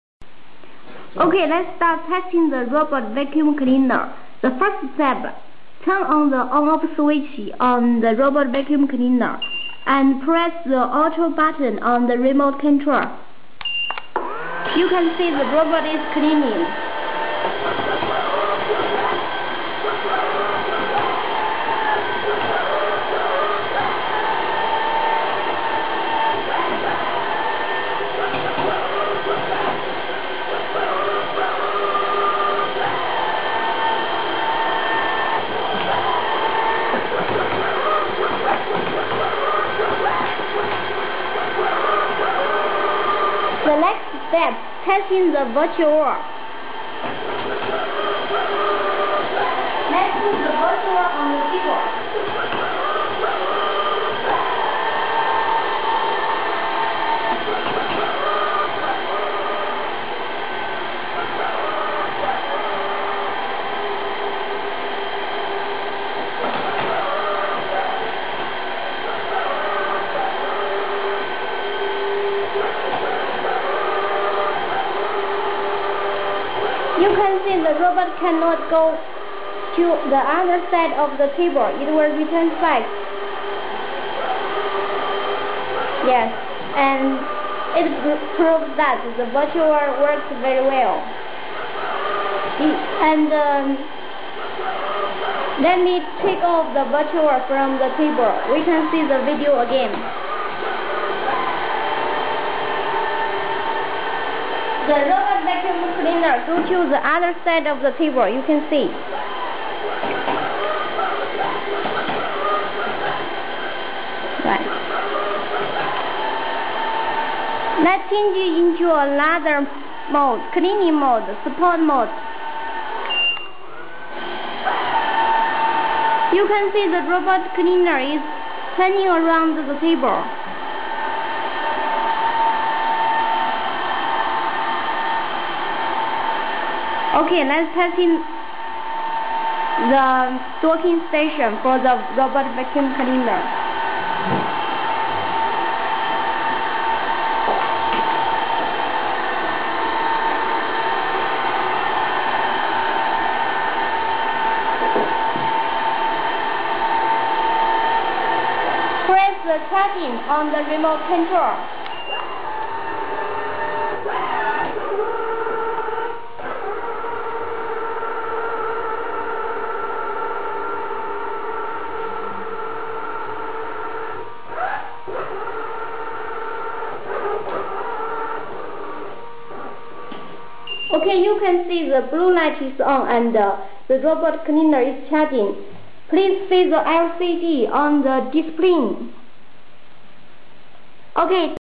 Robot Vacuum Cleaner - A320 Demo Video
Robot Vacuum Cleaner - A320 Demo and testing stair avoidance.